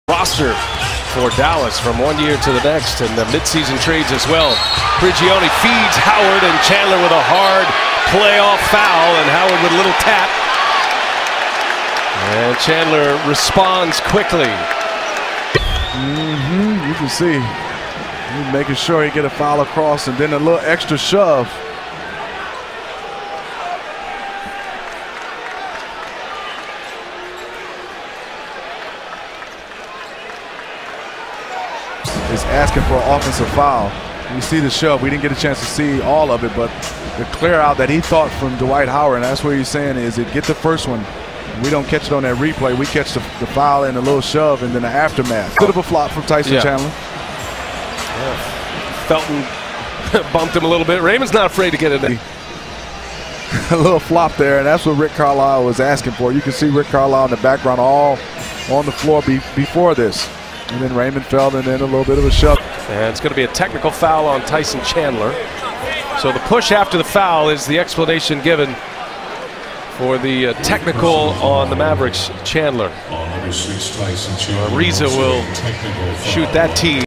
NBA, playoff 2015, Rockets vs. Mavericks, Round 1, Game 2, Move 18, Tyson Chandler, foul
Houston Rockets - Dallas Mavericks, Game 2, 21/04/2015, quarter 2
Tyson Chandler, foul